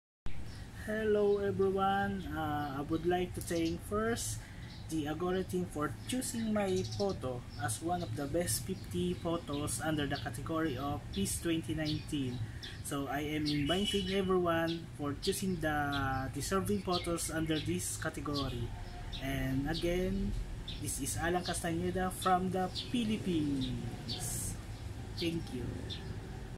Peace2019 Top 50: Allan Castañeda
Meet the photographers behind the 50 best photos in the world of Peace2019. Who will be our next AGORA Hero? Your point of view is unique so vote for which you believe to be the World’s Best Photo of Peace2019!